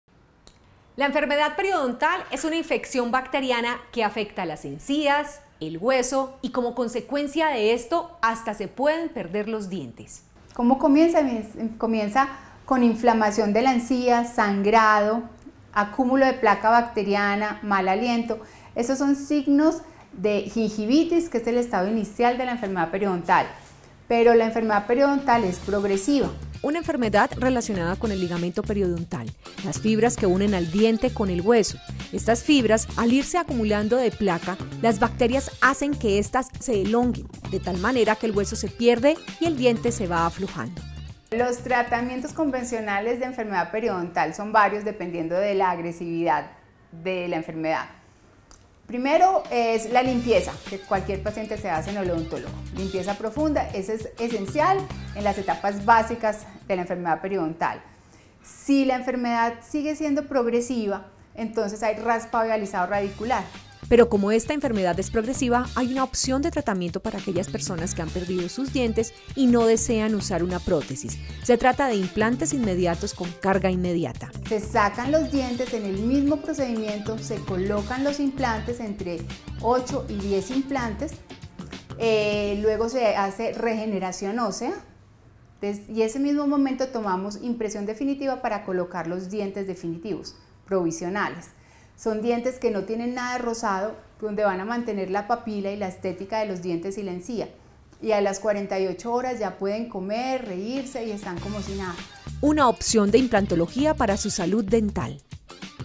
OPCIÓN DE IMPLANTOLOGIA PARA LA ENFERMEDAD PERIODONTAL
La enfermedad periodontal es una infección bacteriana que afecta las encías y el hueso y como consecuencia de esto se pueden hasta perder los dientes
Dra. Patricia Uribe Vargas odontóloga especialista en rehabilitación oral e implantologia.